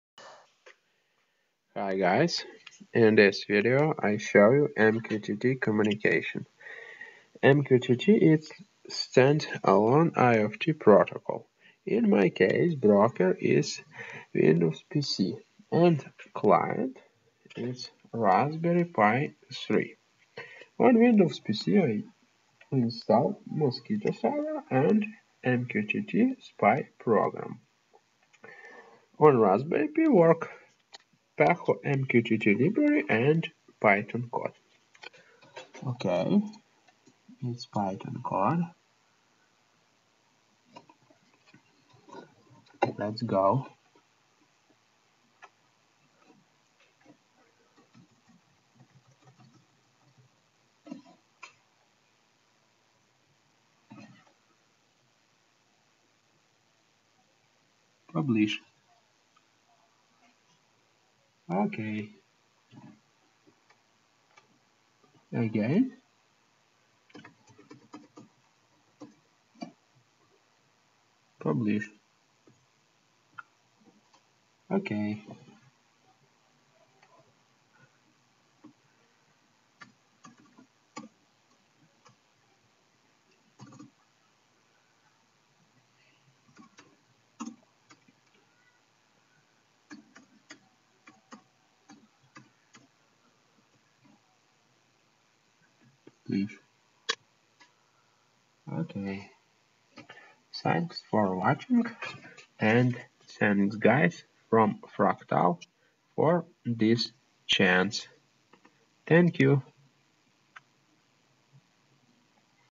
MQTT communicated between Windows PC and Raspberry Pi
On Windows PC run Mosquitto server and MQTT-Spy. Raspberry Pi use paho-mqtt client